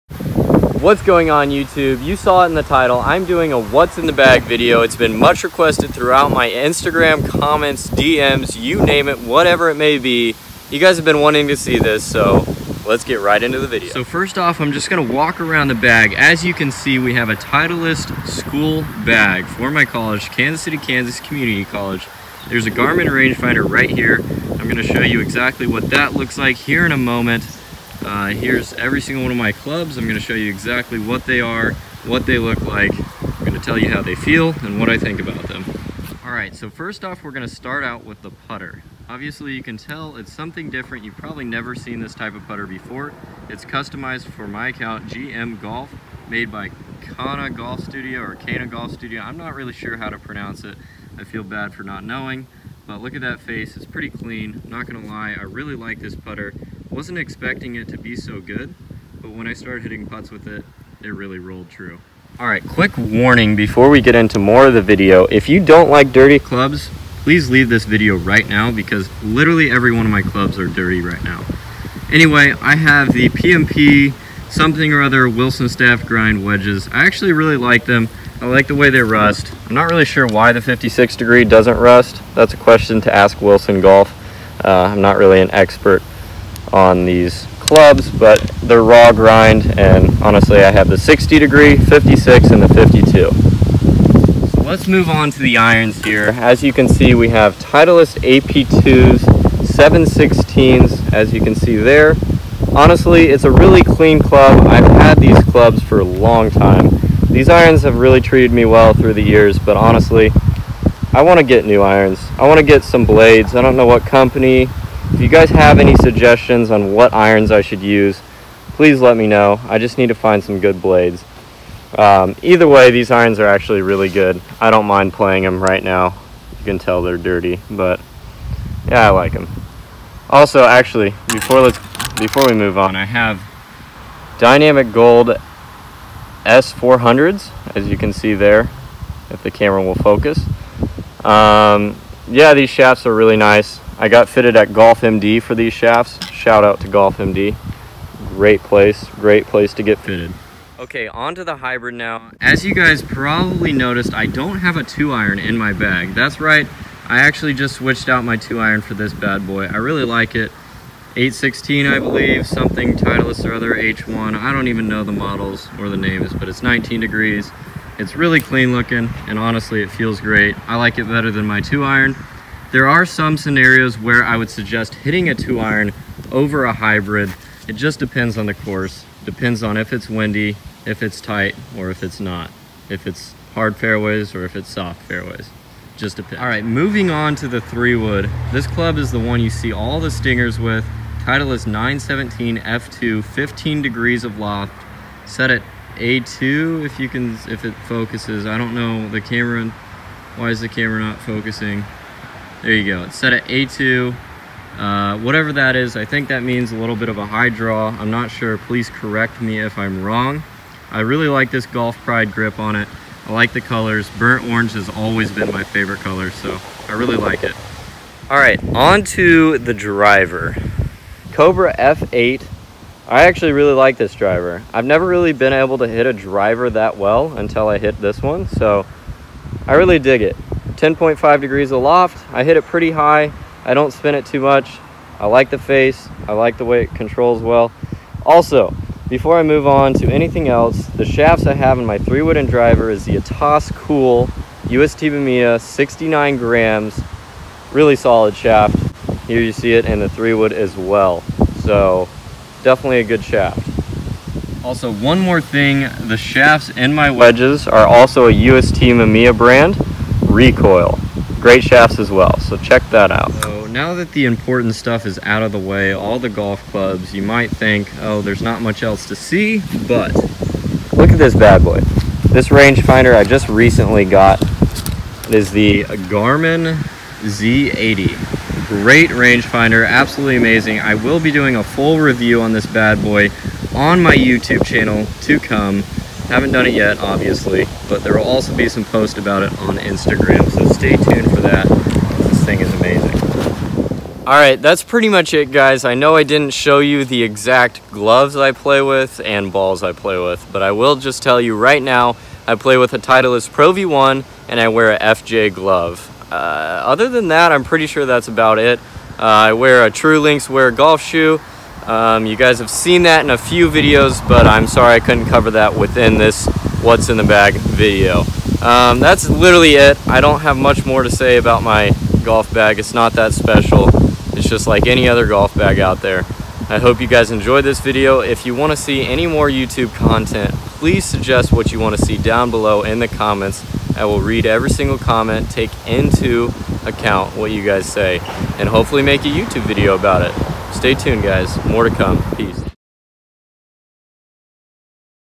What’s In The Bag 2018 - gm golf
Hey guys, lately this has been a much requested video! Considering that, I decided I would make a video on my “What’s In The Bag”.